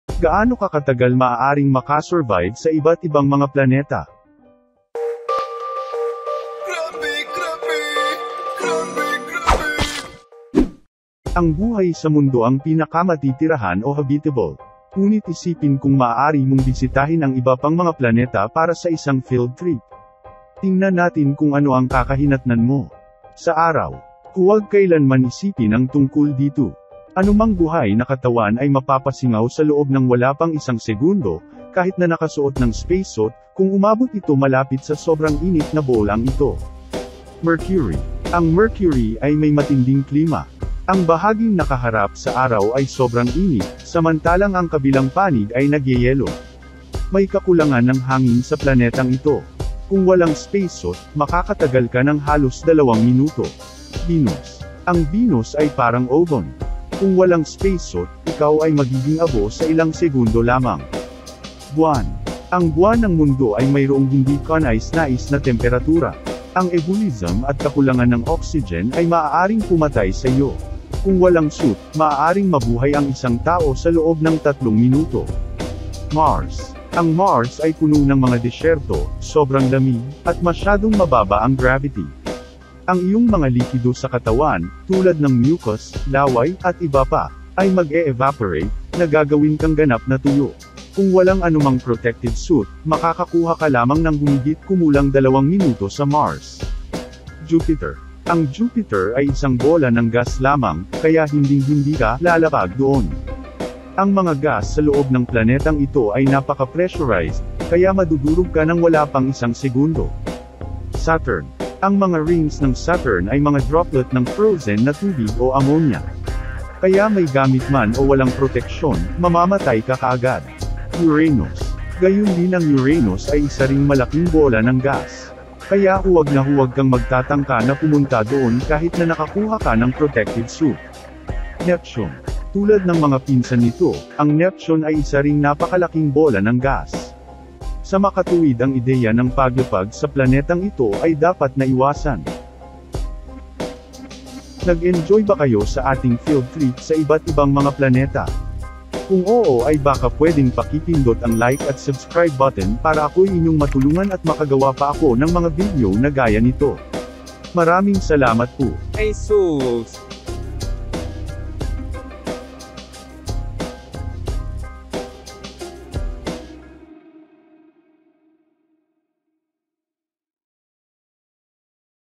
GAANO KATAGAL MAAARING MAKASURVIVE SA IBA'T IBANG MGA PLANETA?
Ang buhay sa mundo ang pinaka-matitirahan o habitable. Ngunit isipin kung maaari mong bisitahin ang iba pang mga planeta para sa isang field trip? Tingnan natin kung ano ang kakahinatnan mo.

You may turn on English or Tagalog captions for your convenience.

0:00 Intro
0:23 Sun
0:37 Mercury
0:52 Venus
1:02 Moon
1:16 Mars
1:36 Jupiter
1:50 Saturn
2:00 Uranus
2:11 Neptune
2:23 Windup